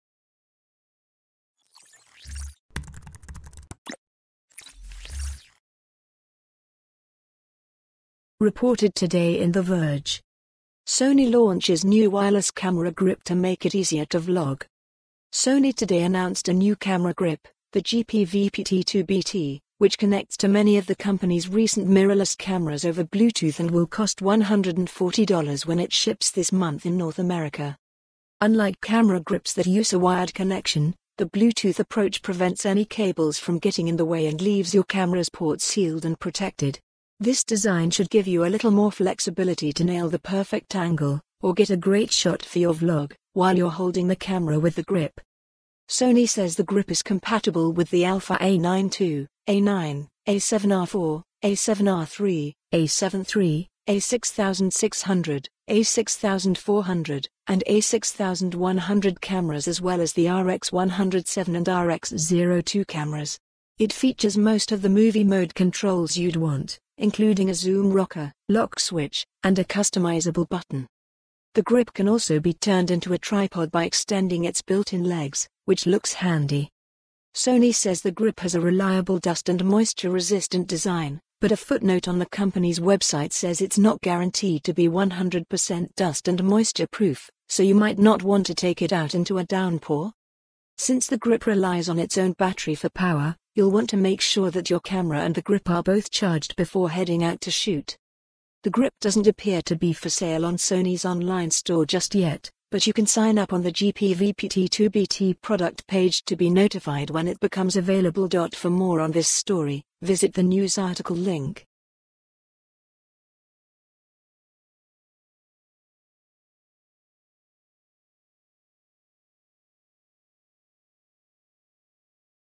Sony launches new wireless camera grip to make it easier to vlog
Sony today announced a new camera grip, the GP-VPT2BT, which connects to many of the company's recent mirrorless cameras over Bluetooth and will cost $140 when it ships this month in North America.

Unlike camera grips that use a wired connection, the Bluetooth approach prevents any cables from getting in the way and leaves your camera's ports sealed and protected. This design should give you a little more flexibility to nail the perfect angle (or get a great shot for your vlog) while you're holding the camera with the grip.

Sony says the grip is compatible with the Alpha A9 II, A9, A7R IV, A7R III, A7 III, A6600, A6400, and A6100 cameras as well as the RX100 VII and RX0 II cameras. It features most of the movie-mode controls you'd want, including a zoom rocker, lock switch, and a customizable button.

The grip can also be turned into a tripod by extending its built-in legs, which looks handy:

Sony says the grip has a "reliable dust and moisture resistant design," but a footnote on the company's website says it's "not guaranteed to be 100 percent dust and moisture proof," so you might not want to take it out into a downpour.

Since the grip relies on its own battery for power, you'll want to make sure that your camera and the grip are both charged before heading out to shoot.

The grip doesn't appear to be for sale on Sony's online store just yet, but you can sign up on the GP-VPT2BT product page to be notified when it becomes available.